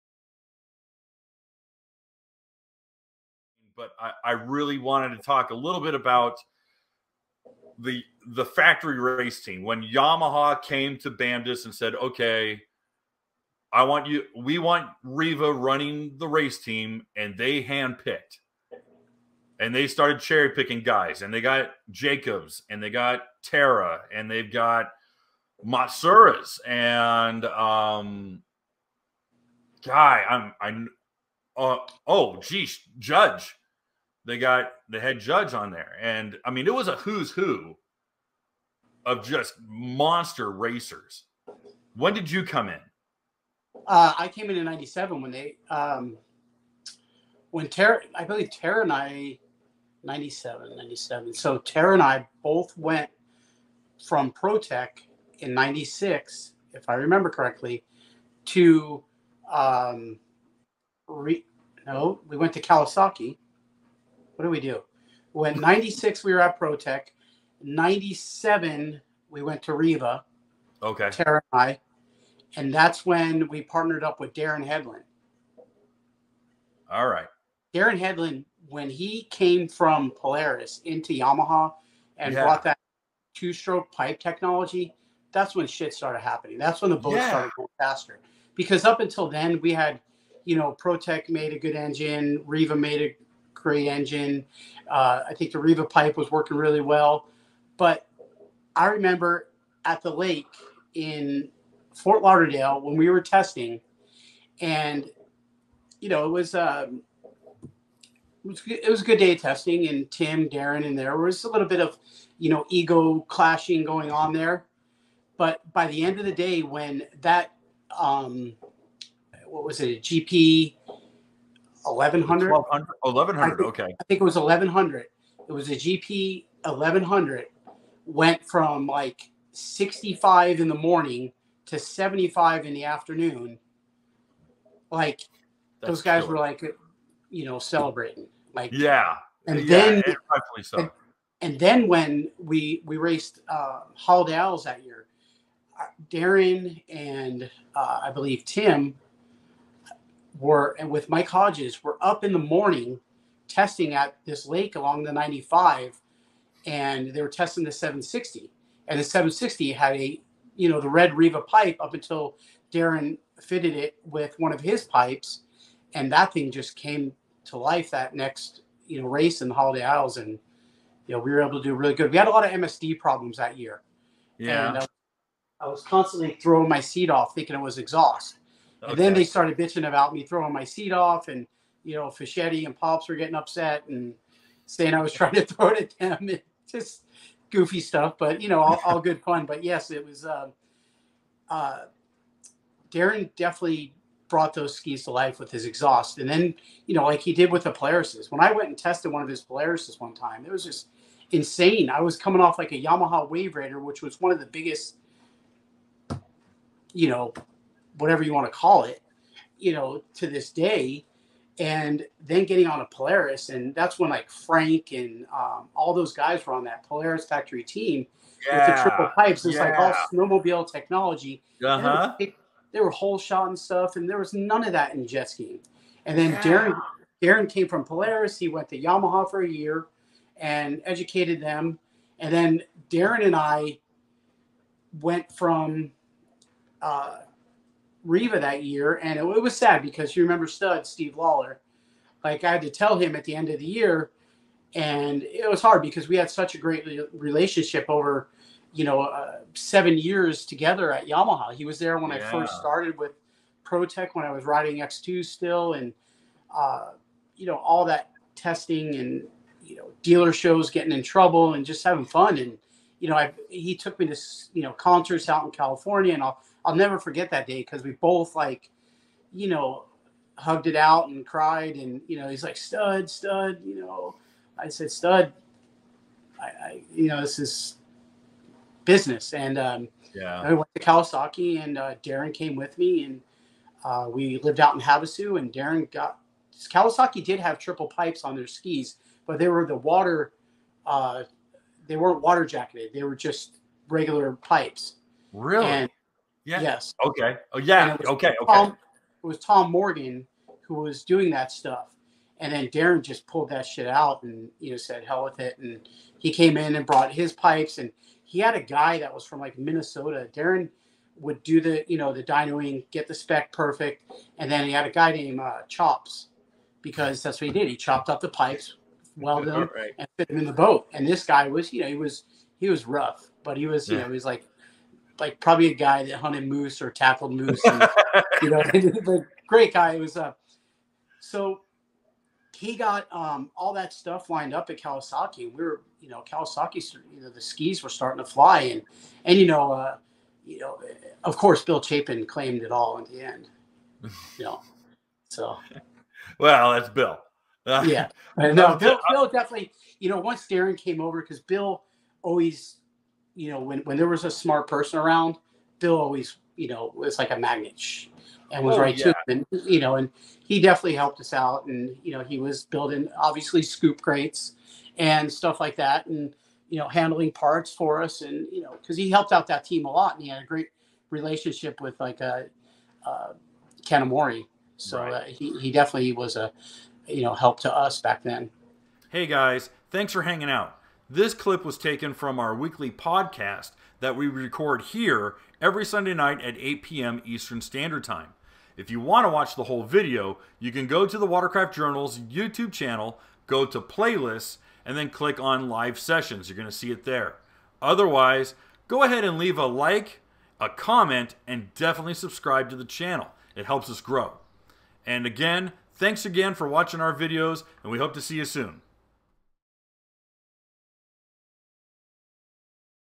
Macc Recalls Working Darren Hedlund at Kawasaki: WCJ Quick Topic
This Quick Topic segment was taken from our full-length podcast titled "Multi-Time IJSBA World Champion Chris MacClugage Refuses to Slow Down" and can be viewed in its entirety